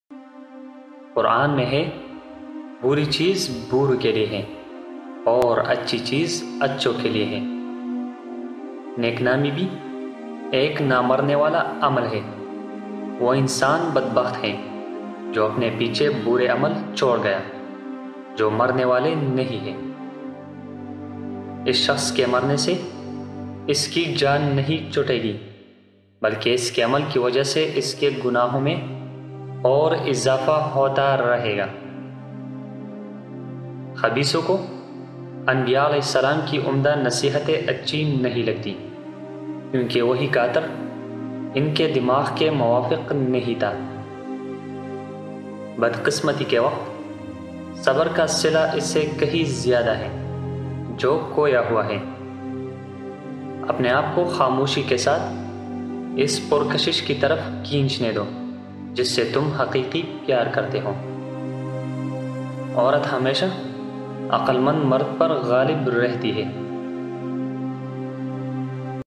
RUMI | Orat Hamaisha Aisy Mard Phar Ghalib Rahti Hai....
In this video rumi. Sunehri Aqwaal by rumi. Urdu Aqwaal e zareen. Islamic Aqwaal.
Design and Publish by Syed Kamran studio (ekhlaq).
Voice: Syed kamran Qureshi.